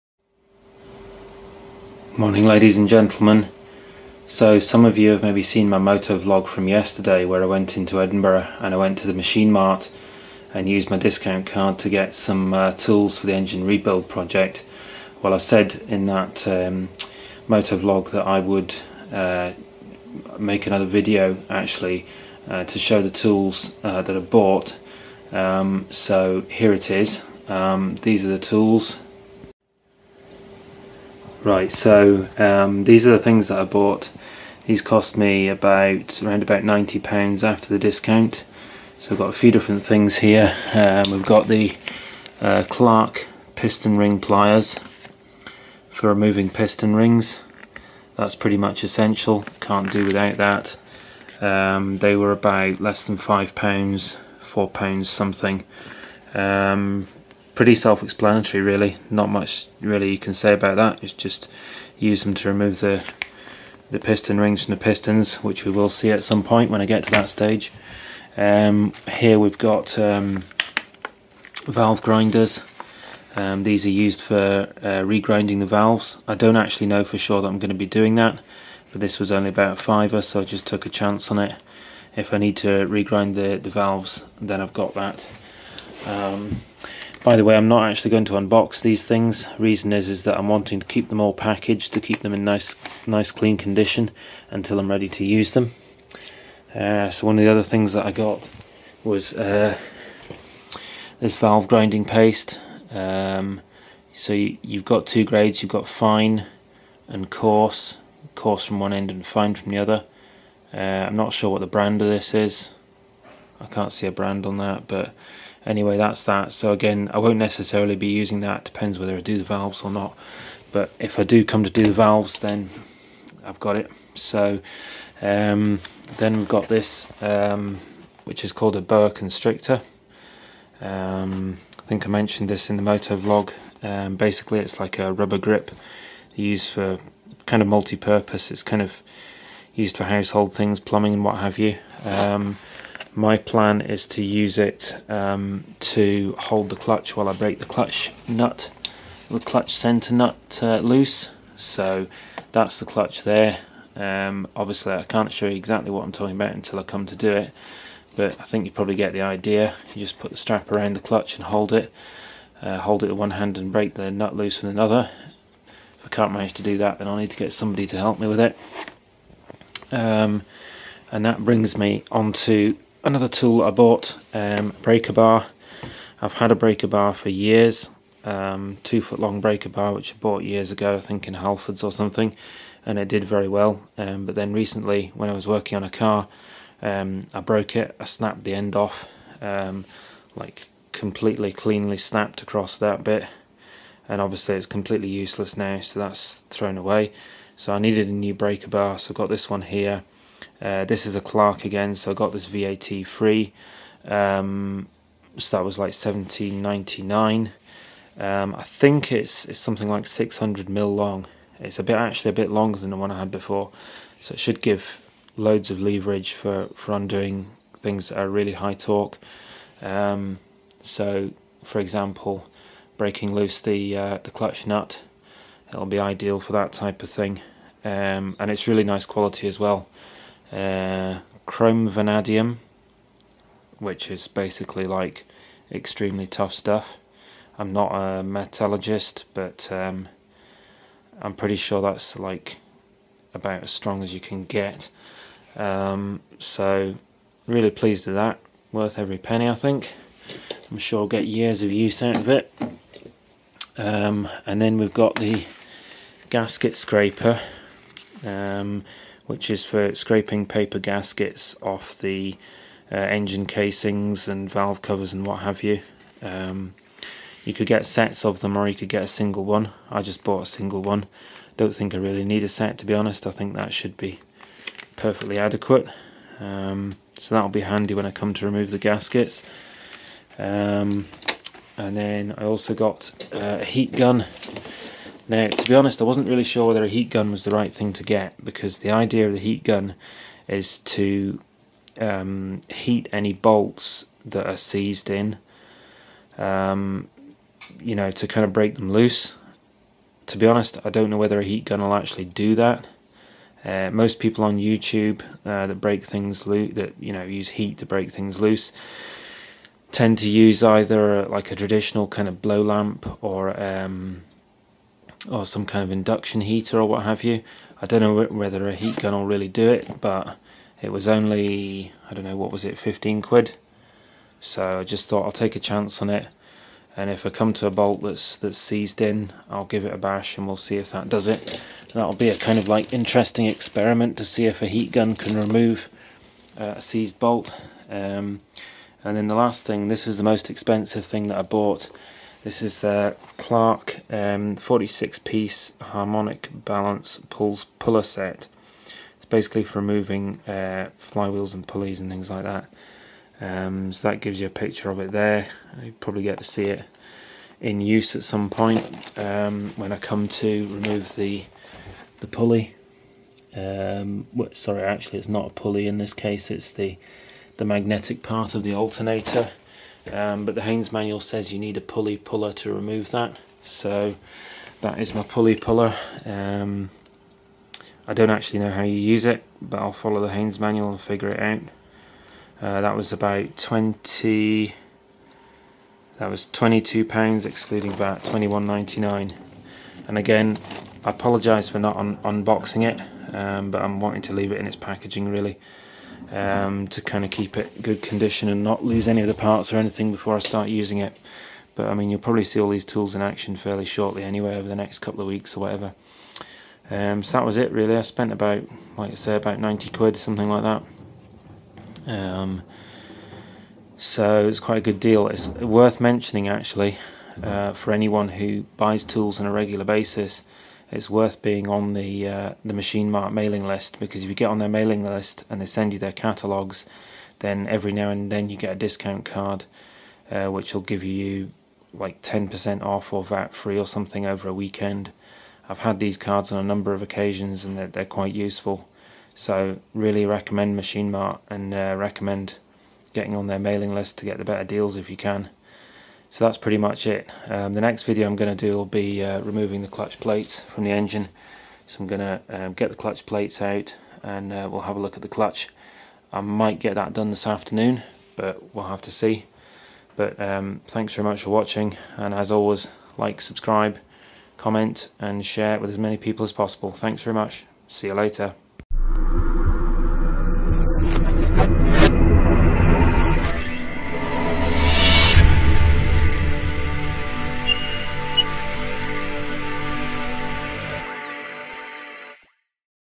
New Tools from Machine Mart for the Engine Project
Here are the new tools that I bought from Machine Mart yesterday, for the VFR800 engine rebuild project.